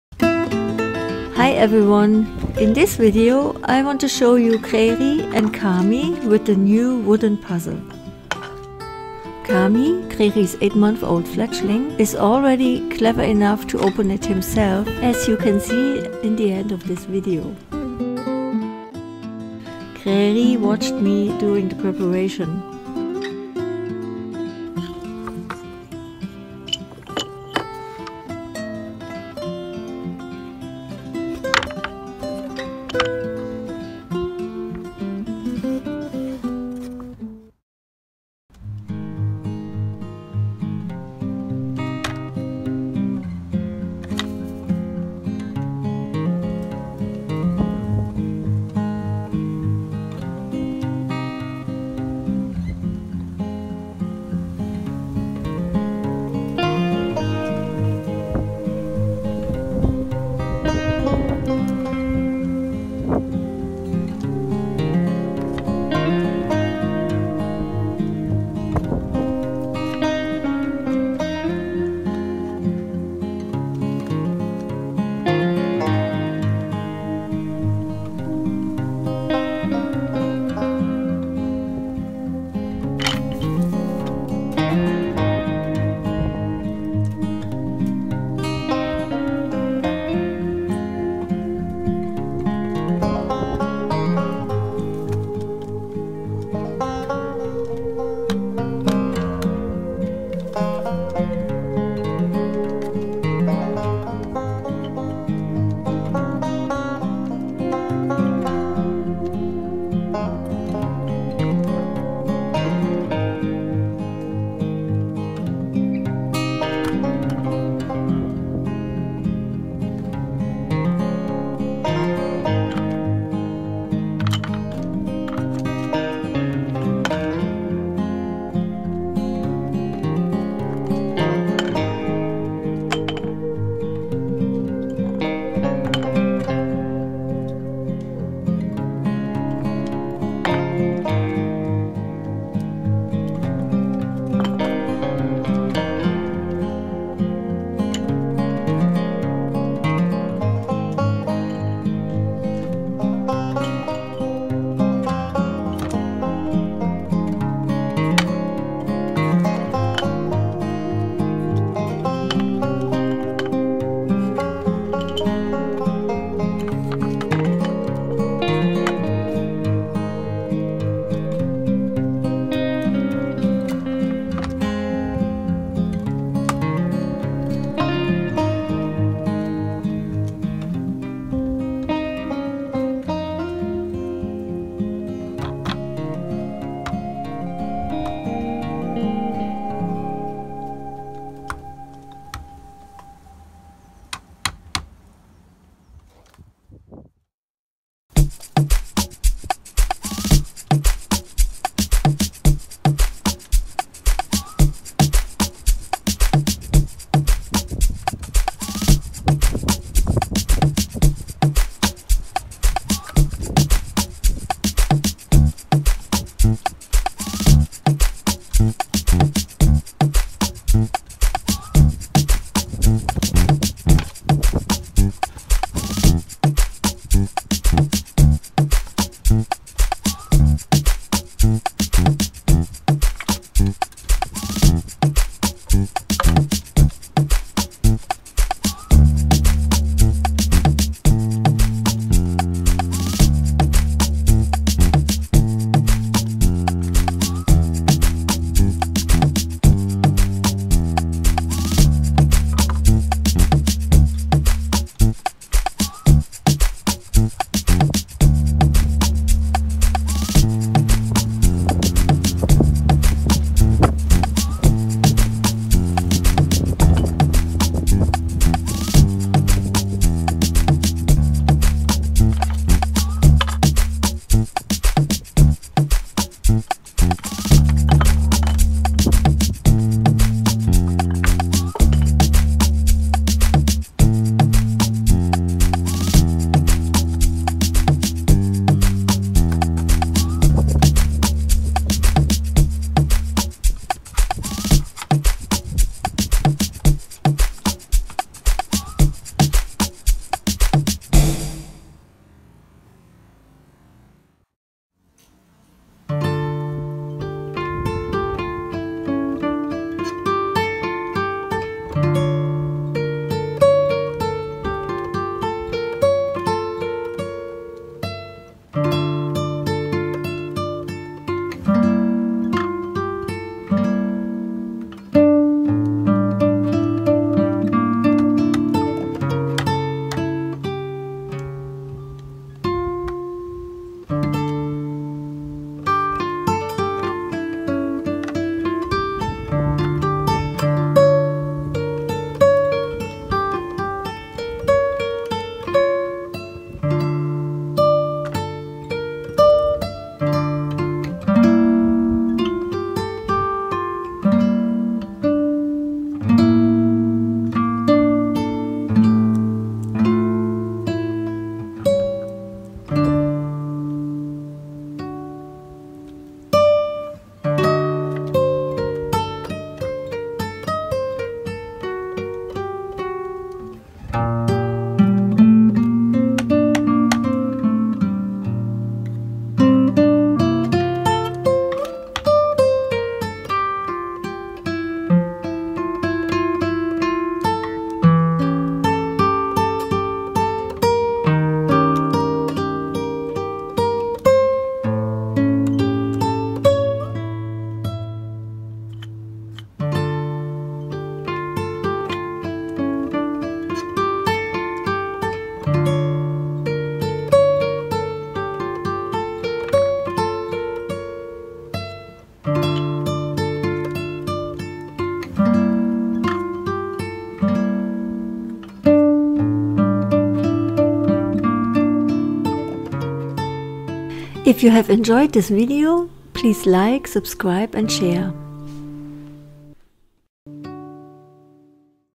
Wild Crows Solving Wood Puzzle
In this video you can see Kräri and his 8 month old  fledgling Kami solving a wood puzzle. In the first part of the video Kräri opens the puzzle and Kami is eager to help. In the second part Kräri opens the puzzle alone and in the third part Kami opens it alone.

Video by Alexandra Rösch and Florian Zeitz